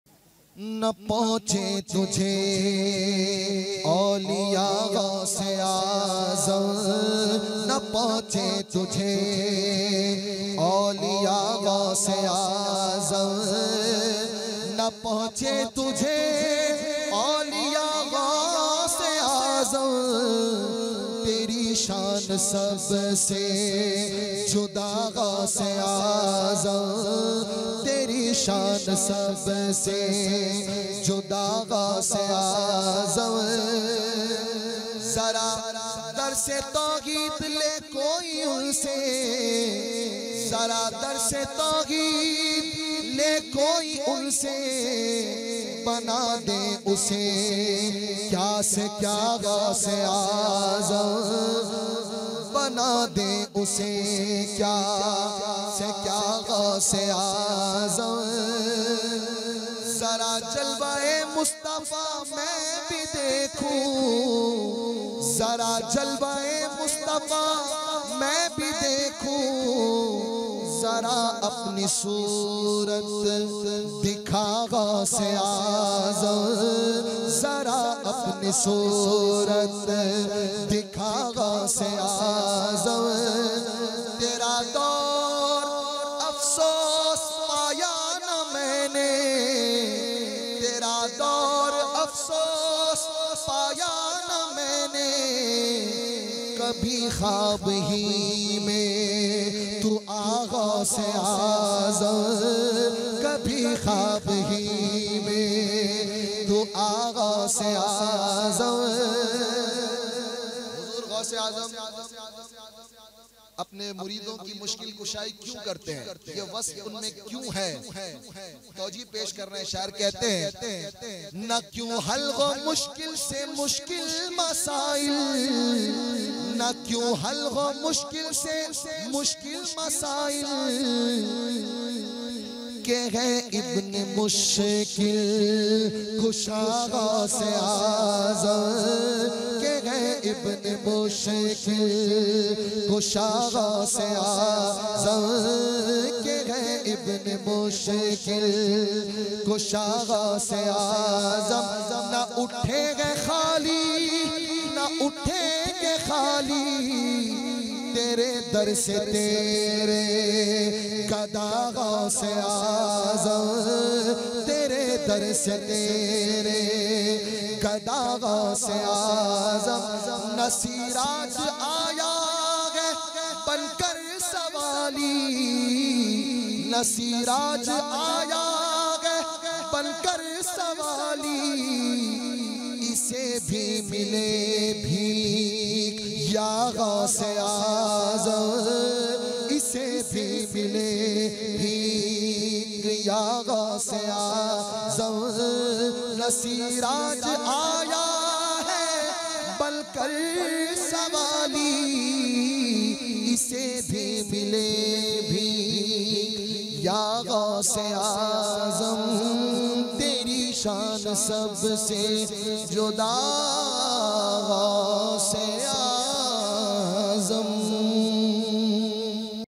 New Manqabat 2023 || Manqabate Gause Aazam || Allama Syed Fazil Miyan Ashrafi | 25-02-2023 || Morbi
Topic : Manqabate Gause Aazam
Orator : Allama Sayyed Fazil Miyan Ashrafi Maysur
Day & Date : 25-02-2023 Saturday
Place : Morbi, Gujarat India

Muhammed Jafar Hashmi
Una Dist Gir Somnath Gujarat India